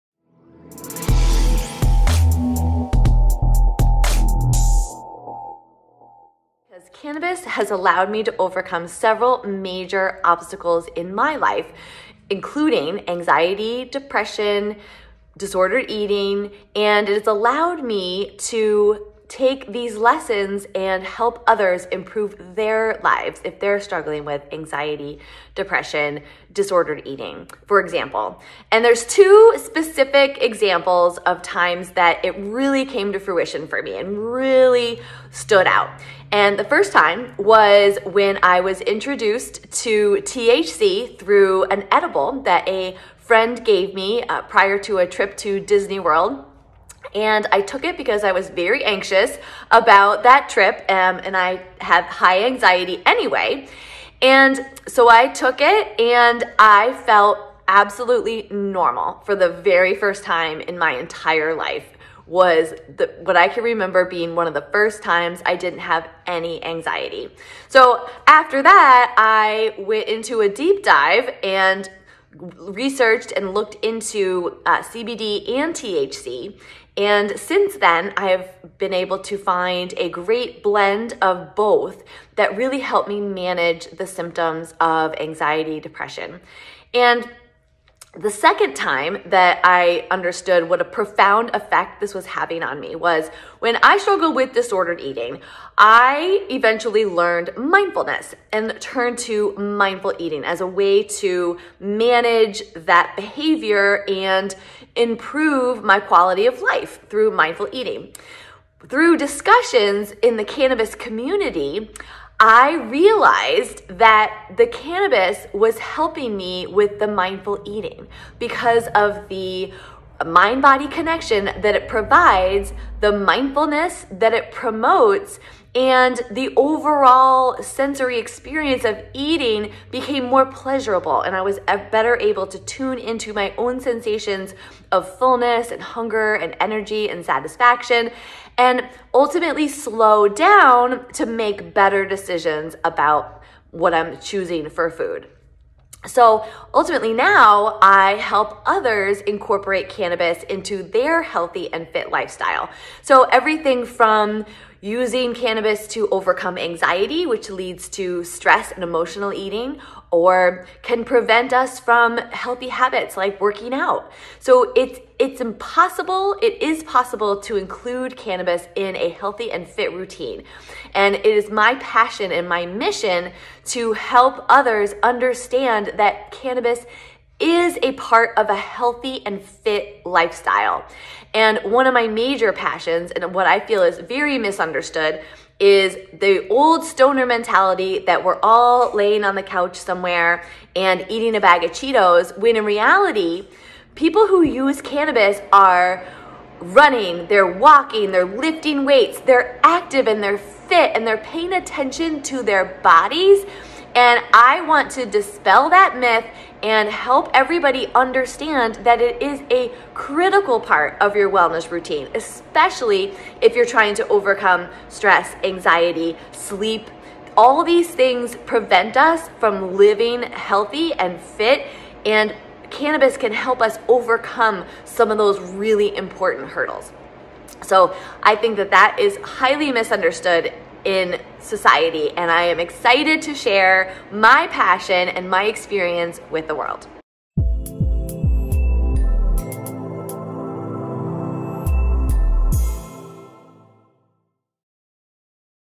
Beth Jasinski: Redefining Health with CannaFit
Beth Jasinski has transformed her personal struggles with yo-yo dieting, disordered eating, and motivation into a mission to help others. By incorporating cannabis into her routine, she has successfully managed her anxiety and depression, combining it with strength training. With 15 years of expertise in fitness and nutrition, Beth now assists others in integrating cannabis with physical activity to optimize health outcomes, promote mindful eating, and encourage purposeful movement.

As a certified cannabis coach, personal trainer, and nutrition coach, Beth has pioneered the "CannaFit" program. This innovative 12-week online course is designed to synchronize fitness, nutrition, and cannabis use, challenging the traditional stoner stereotype and proving that cannabis can indeed be a component of a vibrant, health-focused lifestyle.